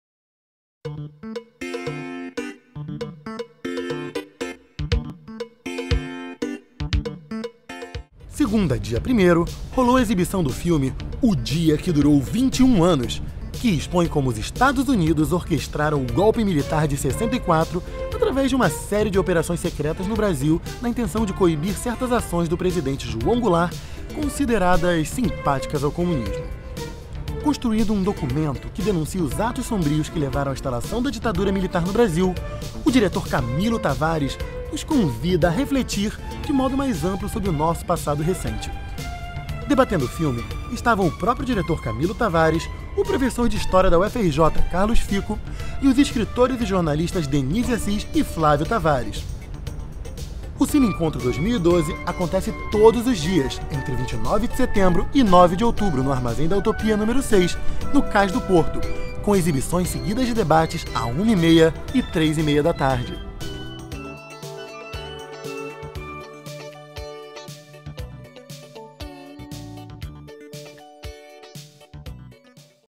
2012 - O Dia que Durou 21 Anos - Debate no Cine Encontro
Segunda, dia 01/10, contou com a exibição do filme " O dia que durou 21 anos", que mostra um retrato da influência do governo norte-americano na imposição da ditadura militar na década de 60.

O debate contou com o diretor do filme, Camilo Tavares, o professor de história da UFRJ  Carlos Fico, e os escritores e jornalistas Denise Assis e Flávio Tavares. A mesa foi mediada pelo jornalista e crítico de cinema Luiz Carlos Merten.